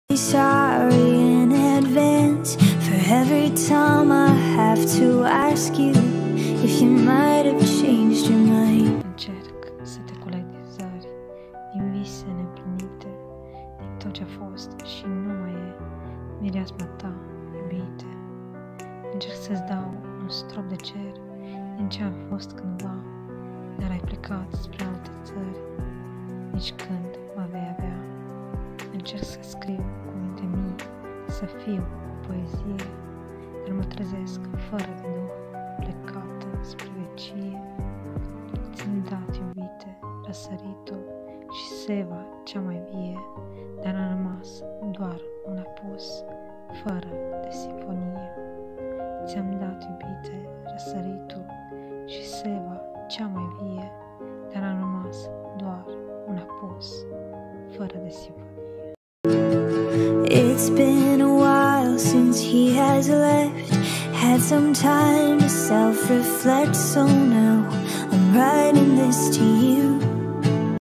POEZIE: INCERC SA TE CULEG DIN ZARI, Poezie de dragoste
Aceasta poezie:INCERC SA TE CULEG DIN ZARI: este una dintre poeziile mele scrise in acest an! Poezie de dragoste!

Scrieti si voi poezii? Va astept cu comentarii..